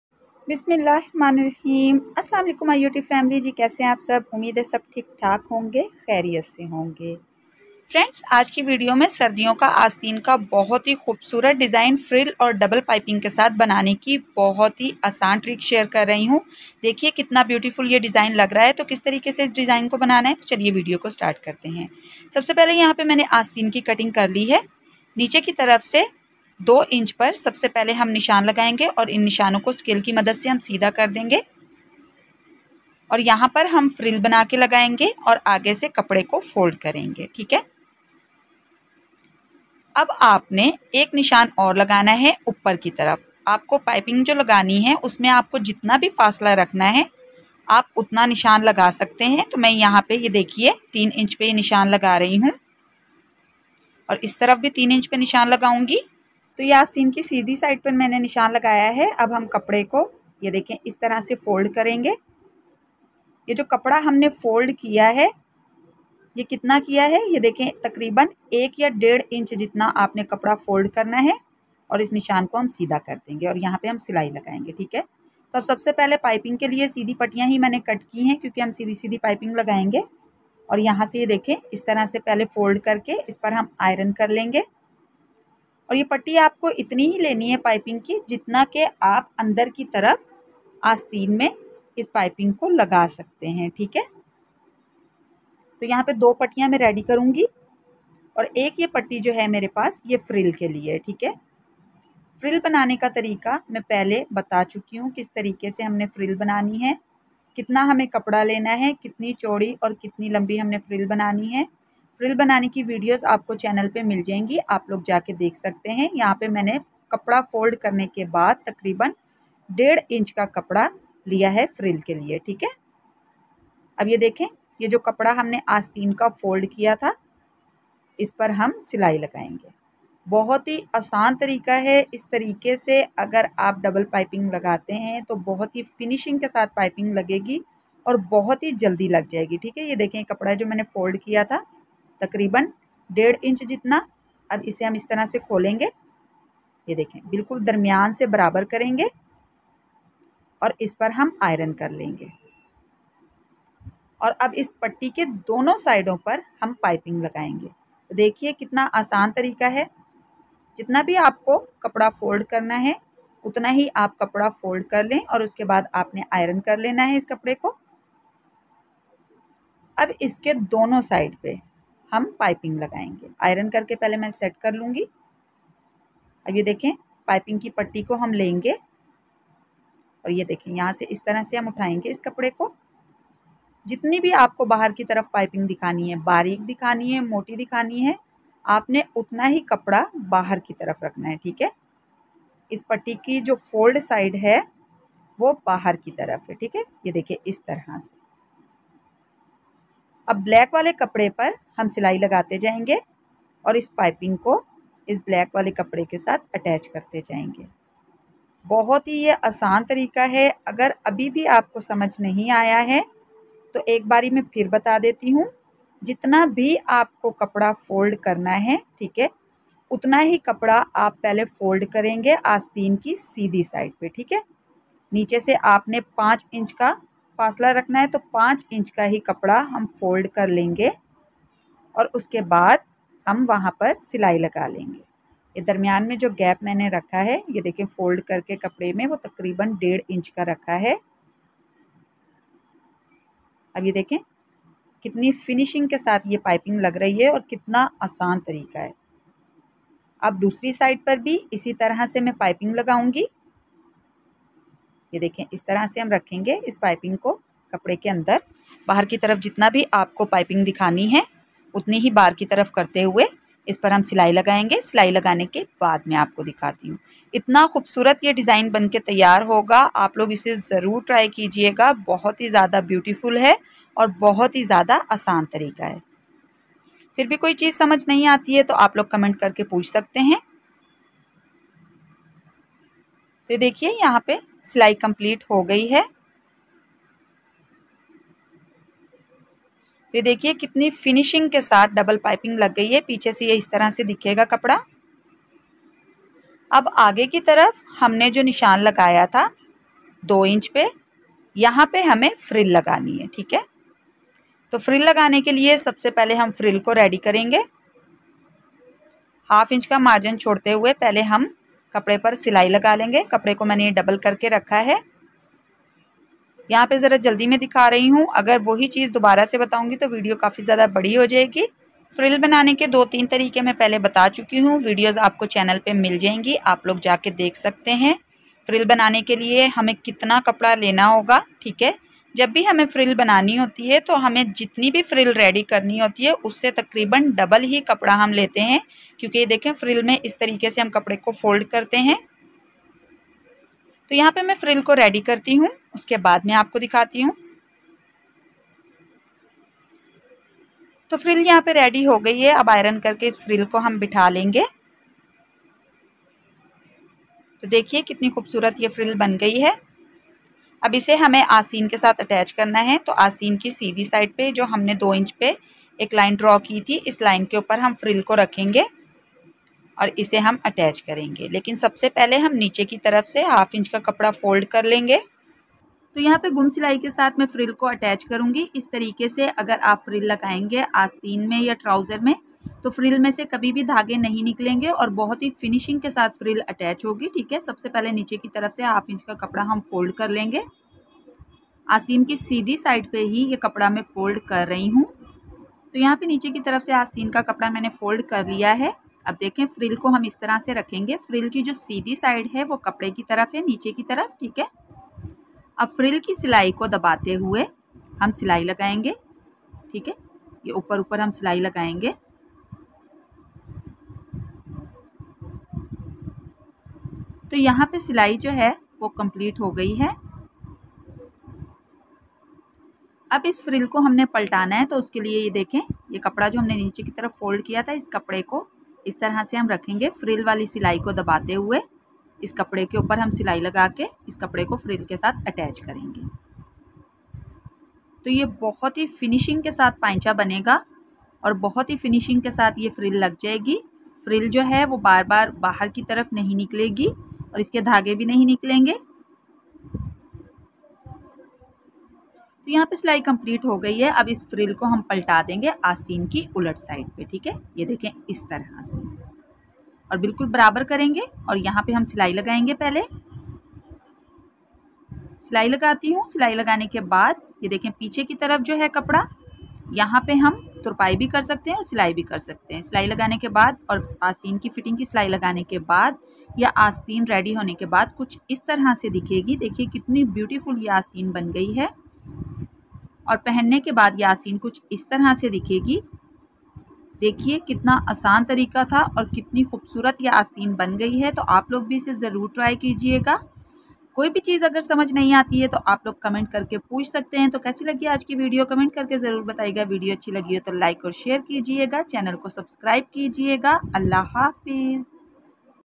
Winter Special New Sleeves Design Cutting and stitching
aj ki video main share kron ge beautiful winter sleeves design trends sleeves design cutting and stitching frill sleeves double piping sleeves design